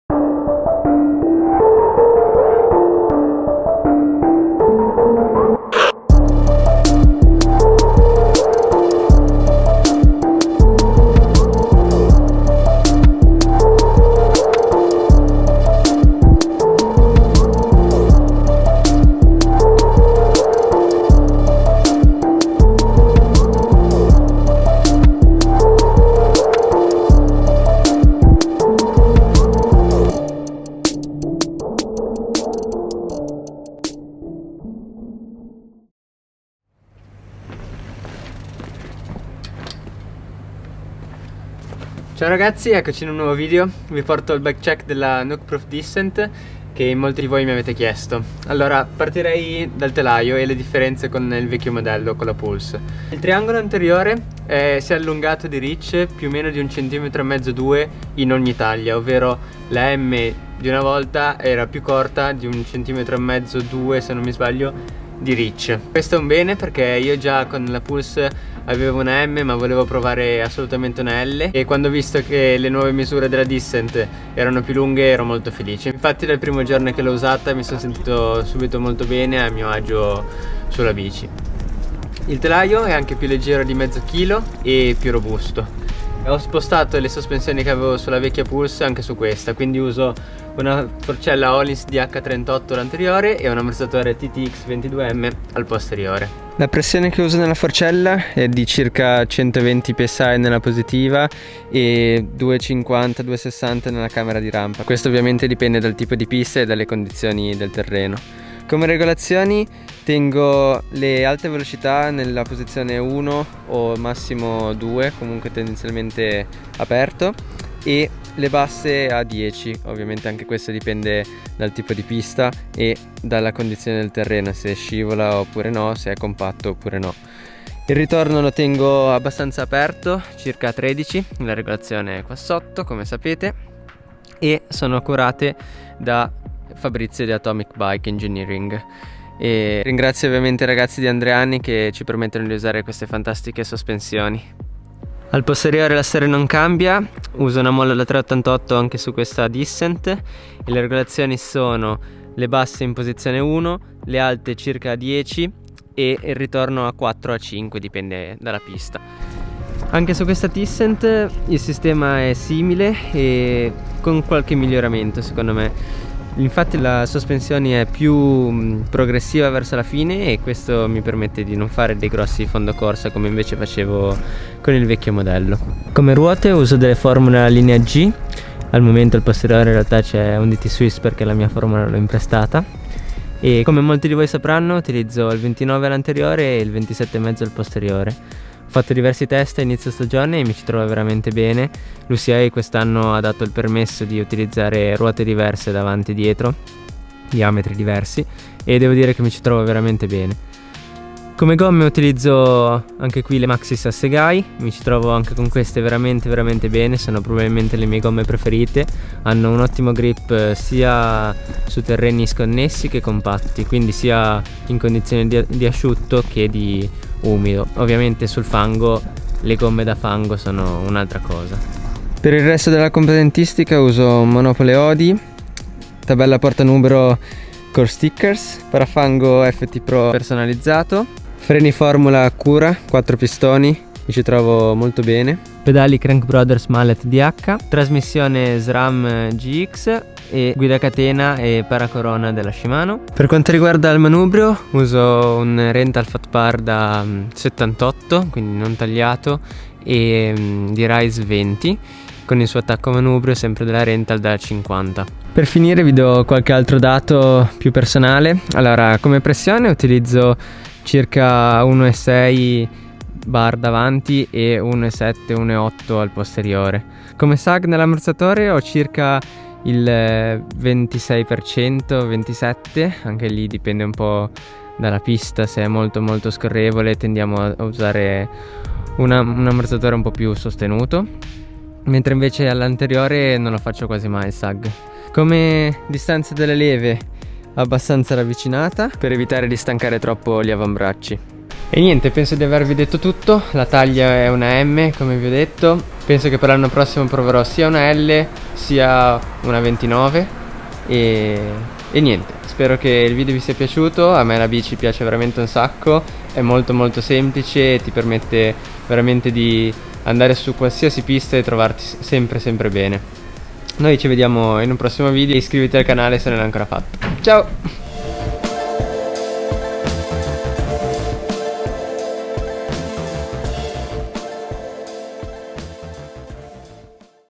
My NUKEPROOF DISSENT - Bike Check
Ecco finalmente il bike check della bici che mi ha accompagnato da metà stagione in avanti, la Nukeproof Dissent! Un bel salto in avanti rispetto alla vecchia Pulse.